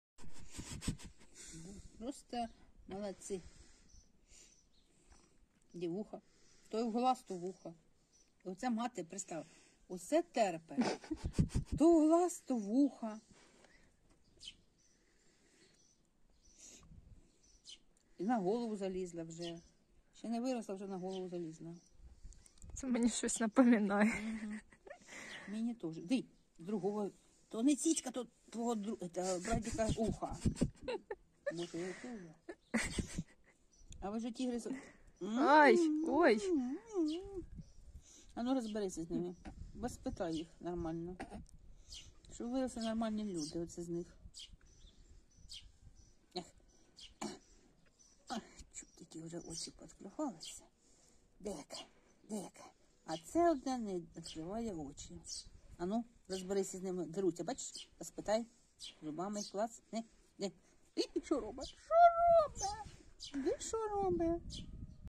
Кішка мама та її кошенята з веселими коментарями / мати усе терпе/ Cat mom with kittens
Кішка мама корме та виховує кошенят.